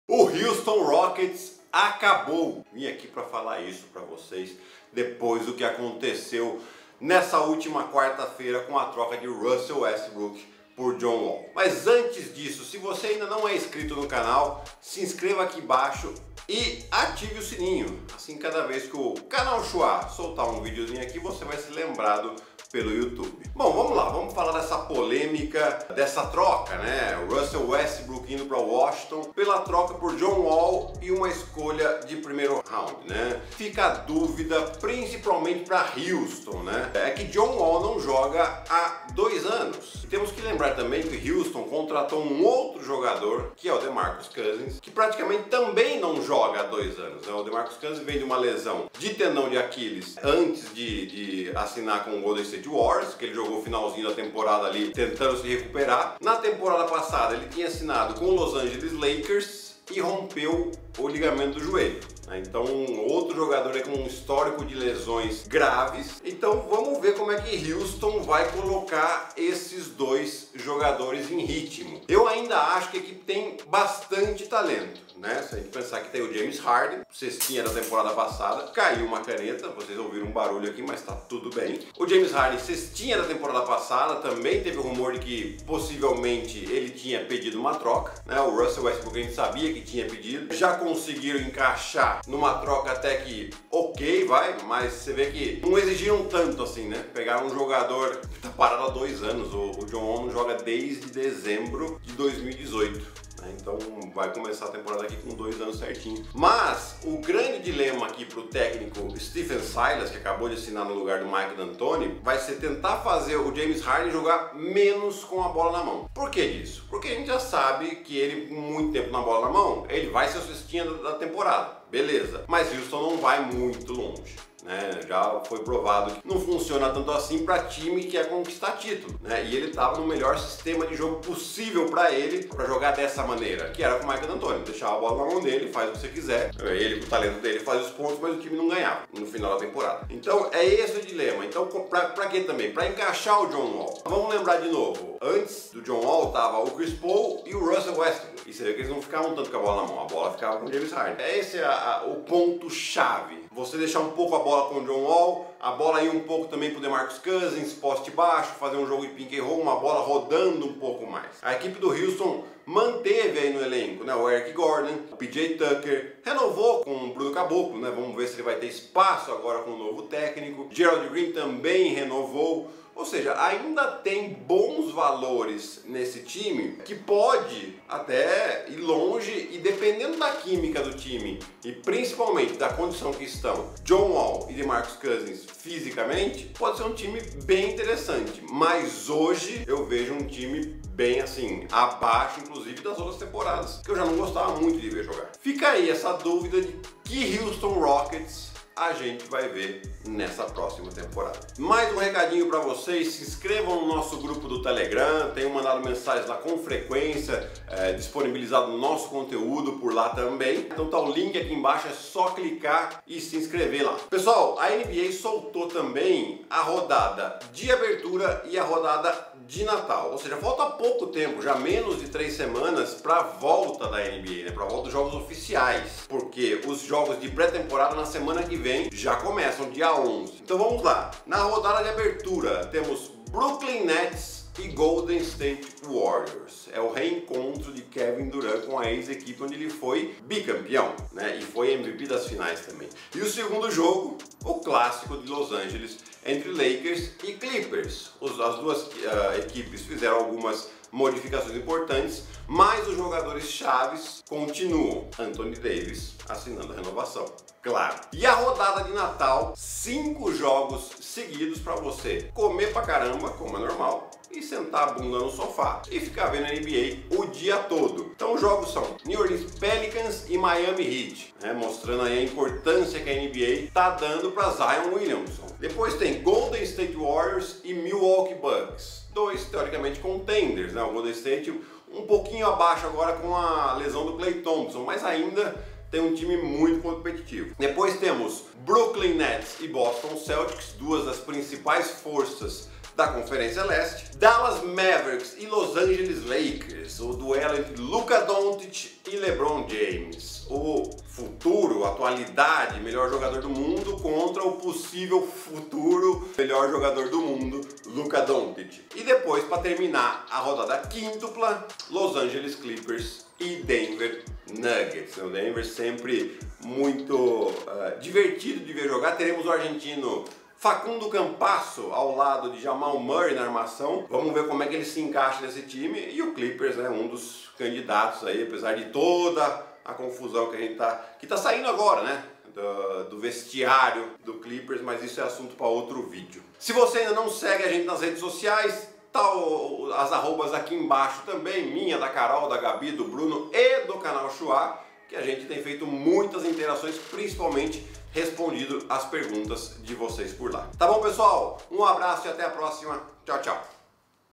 O HOUSTON ROCKETS ACABOU
Na ultima quarta feira o Houston Rockets trocou Russell Westbrook por John Wall e uma escolha de primeira rodada de draft com o Washington Wizards. Com isso, houve uma verdadeira revolução no Texas. Com a saída de Mike D’antoni, o time agora deve encontrar uma maneira diferente de jogar, já que contratou também o povo DeMarcus Cousins. Será que James Harden vai abrir mão da bola pelo menos um pouco agora.
Temos também os jogos de estréia da NBA, onde Kevin Durant vai enfrentar o ex time Golden State Warriors.
Além disso, teremos a tradicional rodada de Natal com 5 jogos seguidos!
Acompanhe as novidades do mundo da NBA, que já está próxima de voltar!